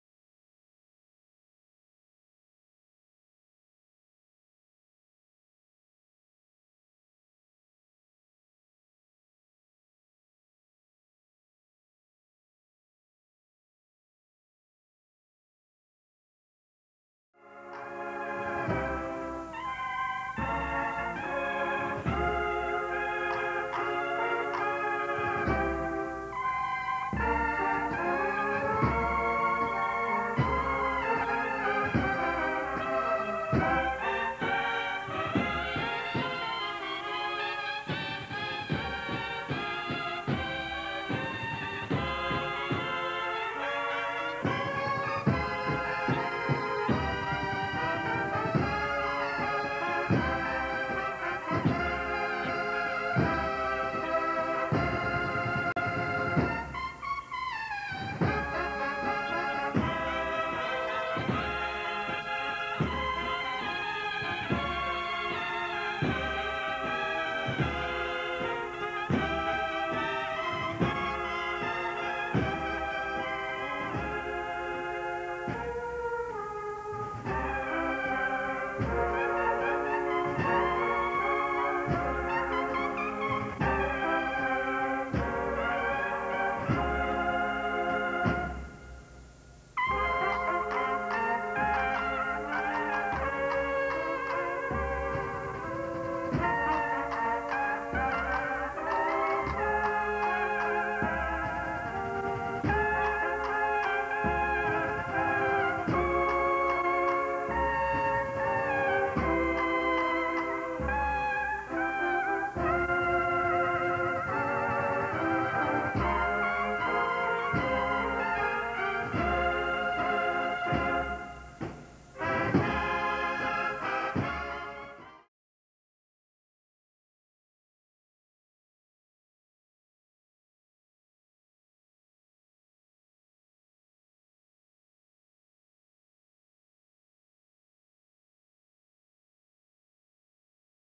SALIDA NAZARENO 400 ANIVERSARIO.HUELVA
Salida de Nuestro Padre Jesús Nazareno de Huelva el día 22 de Octubre de 1983 para recibir la medalla de la ciudad con motivo de la celebración del 400 Aniversario de la fundación de la Hermandad.